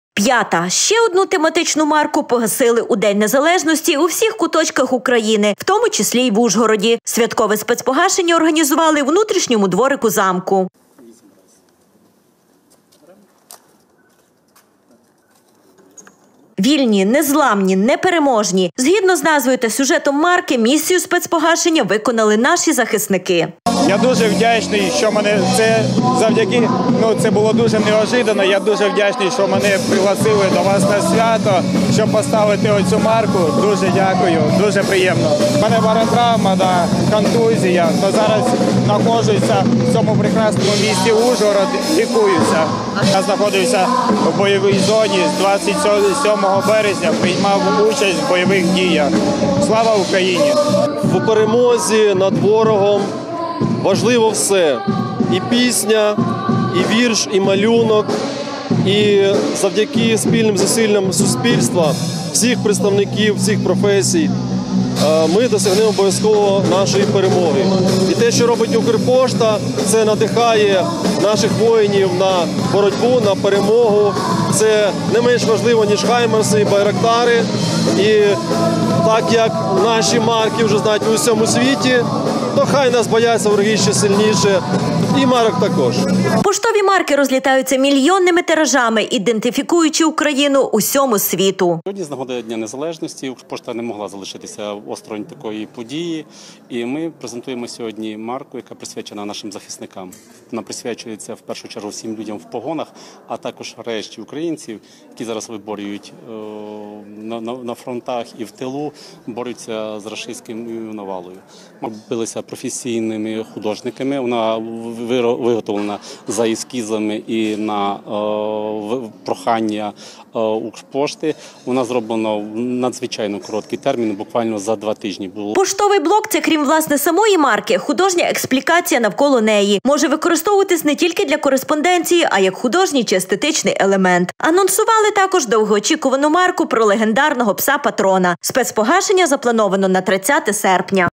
Святкове спецпогашення марки
Поштову  марку «Вільні, Незламні, Непереможні» з нагоди Дня Незалежності  погасили у всіх куточках  України. В  Ужгороді -також. О 15 годині у внутрішньому дворику Закарпатського  Обласного краєзнавчого музею імені Тиводара Легоцького- це Ужгородський замок- урочисто доручили справу нашим захисникам. До слова, це вже п’яте спецпогашення воєнного часу. Подію не оминули наші журналісти. П’ята! Ще одну тематичну марку погасили У День незалежності у всіх куточках України, в тому числі й в Ужгороді. Святкове спецпогашення організували у внутрішньому дворику замку.  «Вільні, Незламні, Непереможні»-  згідно з назовю та сюжетом марки – місію спецпогашення виконали наші захисники.  Поштові марки розлітаються мільйонними тиражами ідентифікуючи Україну усьому світу.  Поштовий блок- це крім власне самої марки, художня експлікація навколо неї. Може використовуватися не тільки для кориспонденції, а як художній чи естетичний елемент. Анонсували і  довгоочікувану марку про легендарного пса Патрона  - спецпогашення заплановано на 30 серпня.